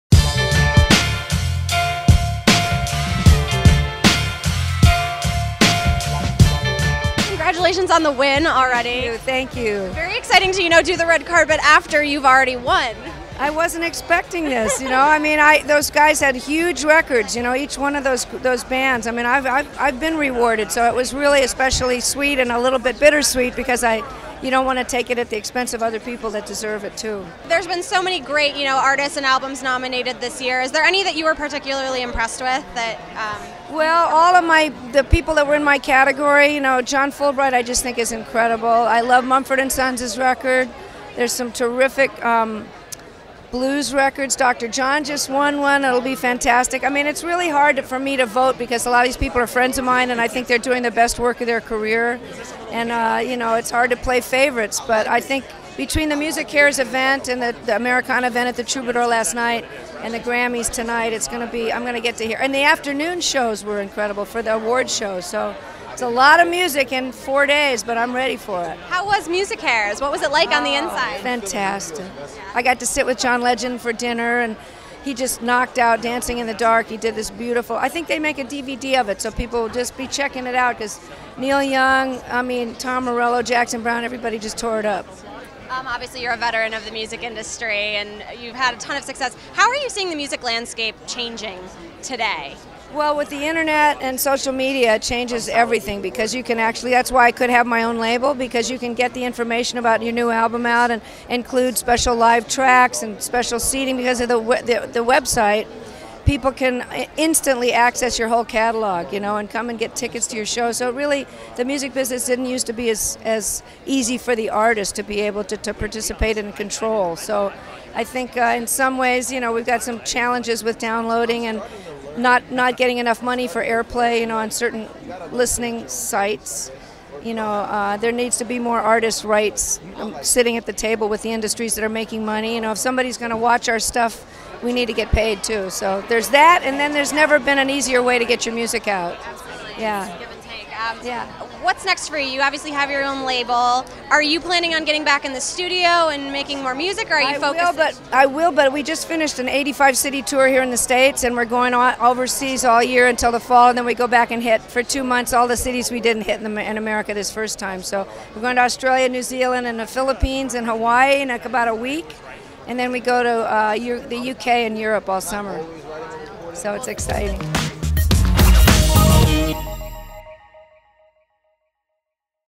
Grammys 2013: Bonnie Raitt on Her 10th Win, the New Frontier of Digital Downloads
Iconic singer/songwriter Bonnie Raitt hits the red carpet following her pre-telecast win for Best Americana Album -- the 10th Grammy of her career and her first win since 1997.